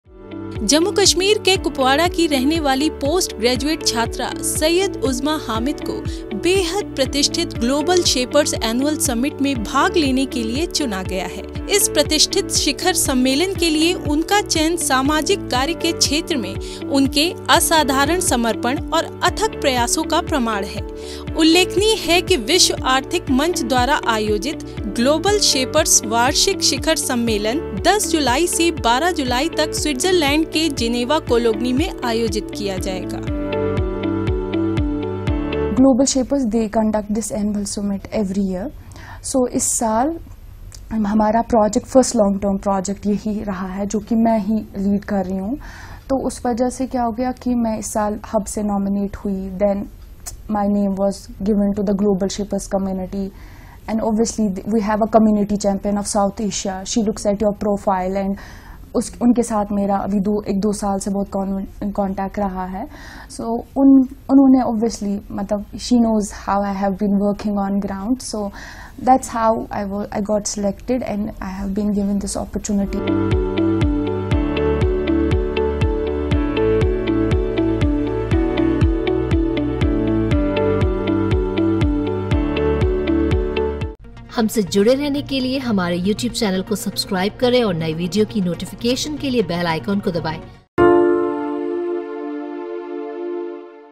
Kashmiri student Syed Uzma Hamid Switzerland में लहराएंगी तिरंगा | WEF Global Shapers Annual Summit
Srinagar, J&K, July 4, Jammu और Kashmir के Kupwara की रहने वाली postgraduate student Syed Uzma Hamid को बेहद प्रतिष्ठित Global Shapers Annual Summit में भाग लेने के लिए चुना गया है। इस प्रतिष्ठित शिखर सम्मेलन के लिए उनका चयन सामाजिक कार्य के क्षेत्र में उनके असाधारण समर्पण और अथक प्रयासों का प्रमाण है। उल्लेखनीय है कि विश्व आर्थिक मंच (डब्ल्यूईएफ) द्वारा आयोजित ग्लोबल शेपर्स वार्षिक शिखर सम्मेलन 10 जुलाई से 12 जुलाई तक Switzerland के Geneva-Cologne में आयोजित किया जाएगा।